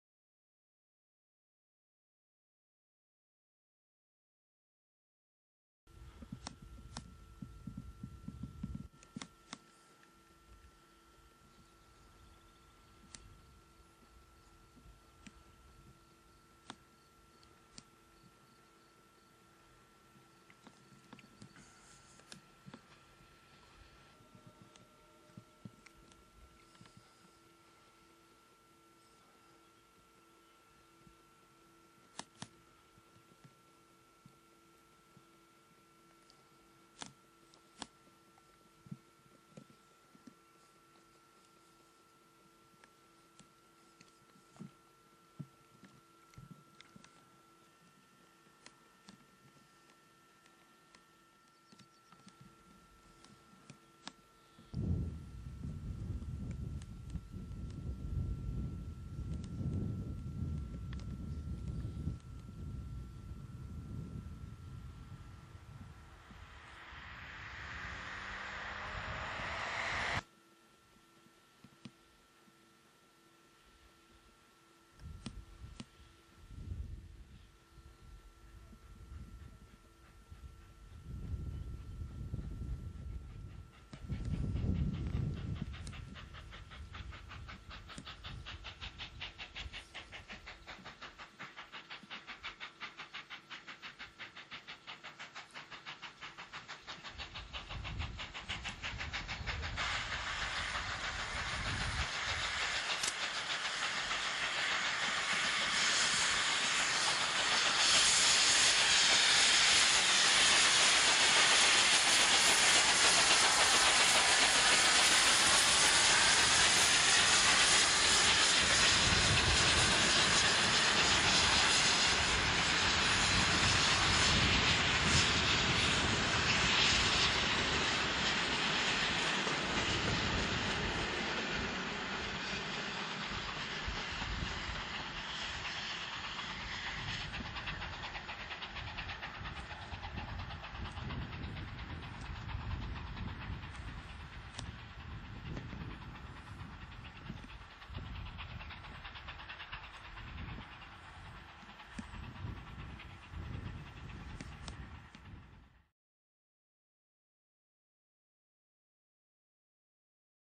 Bahnhof Gurten am 30.6.1991--78 618 kreuzt mit ÖBB-Schienenbus...
Bei einer Sonderfahrt der 78 618 am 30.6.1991 kam es zu dieser Zugkreuzung mit einer aus Braunau kommenden 5081-Garnitur. Der Bahnhof Gurten präsentierte sich damals noch in klassischer Form mit Telegraphenmasten, Flügel-Einfahrsignal und richtigem Güterverkehr. PS.: bitte entschuldigen Sie die recht wackligen Szenen.